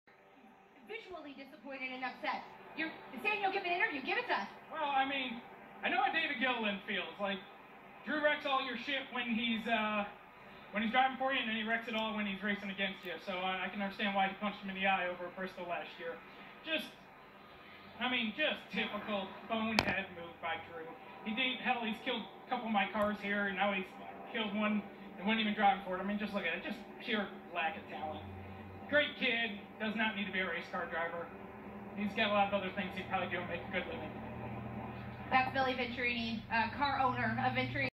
Billy Venturini ROASTS Drew Dollar
Billy Venturini ROASTS Drew Dollar after he took out his driver, Cory Heim in the ARCA Menard series race at Kansas.